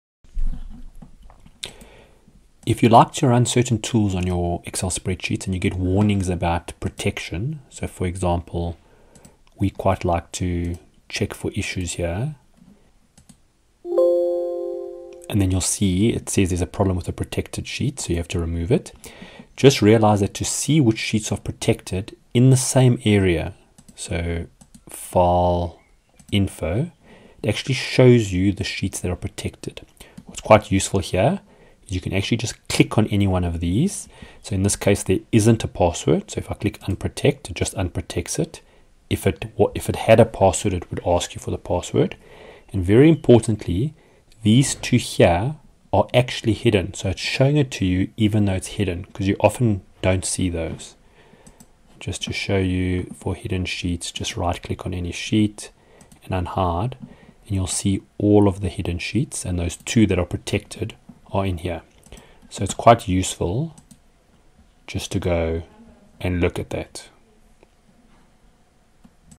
Find all the protected sheets in Excel
00:00 Find which sheets are protected in one go
00:20 Excel lists all the sheets and gives their protection status
00:32 Unprotect all sheets (still individually but at least easier)
00:43 Excel includes list of hidden sheets which are protected

How to get a listing of all the protected sheets in an Excel spreadsheet including any hidden protected sheets.